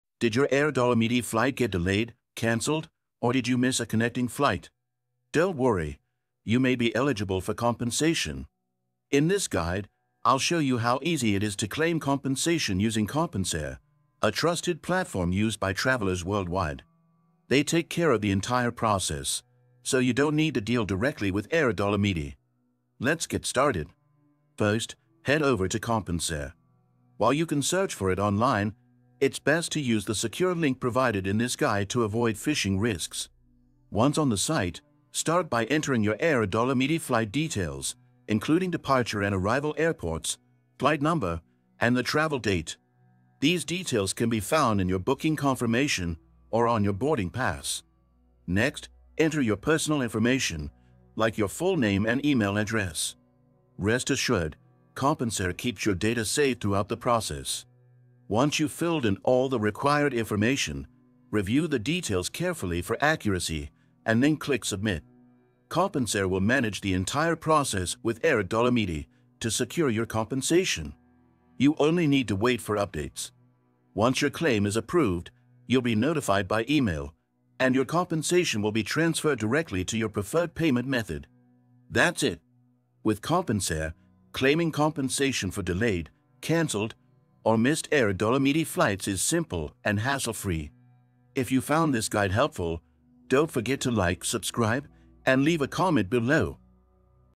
How to Get Compensation for a Delayed or Canceled Air Dolomiti Flight - Easy Guide
Was your Air Dolomiti flight delayed, canceled, or did you miss a connection? You could be entitled to compensation! In this guide, I’ll walk you through the process of claiming compensation for any issues with Air Dolomiti flights by using Compensair, a reliable service that handles everything for you.

Start your claim today by clicking here

Follow along as I explain how to input your flight details, submit your claim, and receive your compensation.